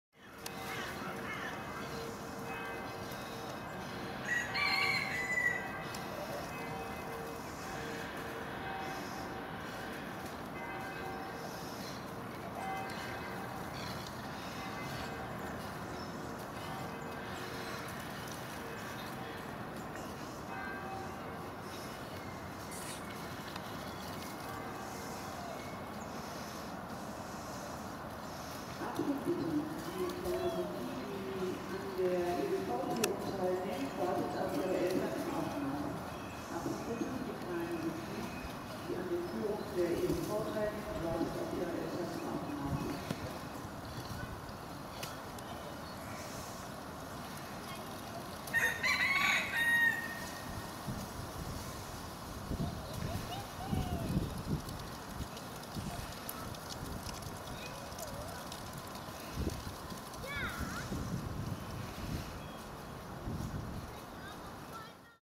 Hirscheber rennt
Ob es an der Kälte liegt, dass der männliche Hirscheber so aktiv ist. Er zeigt im Clip jedenfalls sehr eindrucksvoll eine schnelle Fortbewegungsweise. Es ist nicht die schnellste, denn die Schweineartigen können auch galoppieren.

Whether it is because of the cold that the male deer boar is so active. In any case, he shows in the clip very impressively a fast way of locomotion. It is not the fastest, because the pig-like can also gallop.